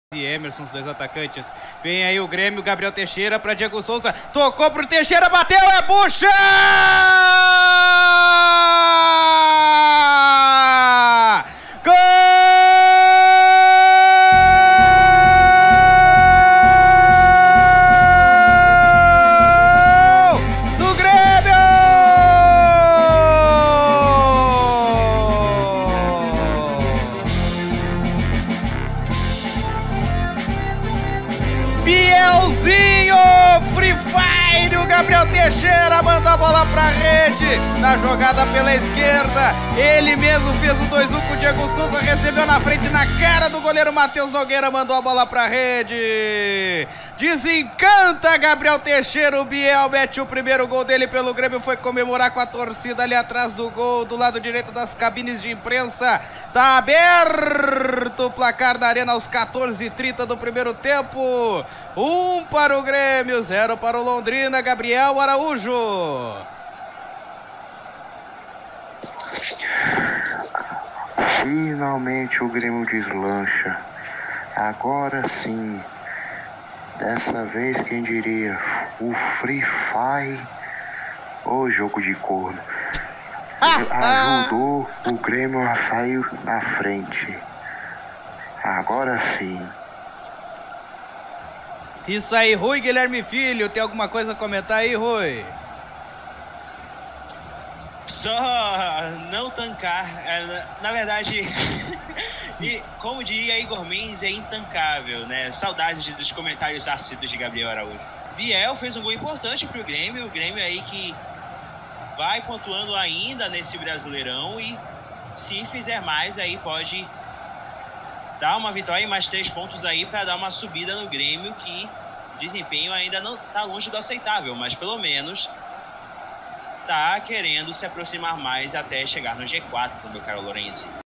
GRÊMIO 1x0 Londrina - Gol - 15ª Rodada - Brasileirão Série B 2022 Narração Vinícius Lorensi
Terceira vitória seguida em casa do nosso Imortal Tricolor nesta dura Série B de 2022. Pra consolidar a sua permanência no G4 por pelo menos mais um tempo!
Jogo realizado em 28/06/2022.

Narração: Vinícius Lorensi